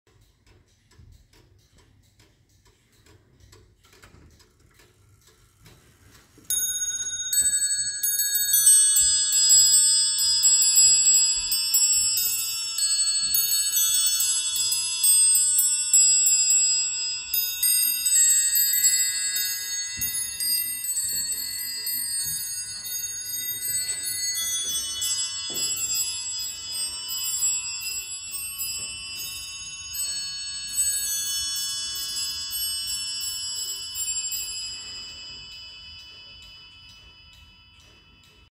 A exceptional large English musical bracket clock with calendar and moonphase of Royal provenance
Look and listen to this exceptional large English bracket clock made for the Dutch market in the 1740's. It strikes the hours and half hours. And it plays one of twelve tunes with thirty hammers on fifteen bells.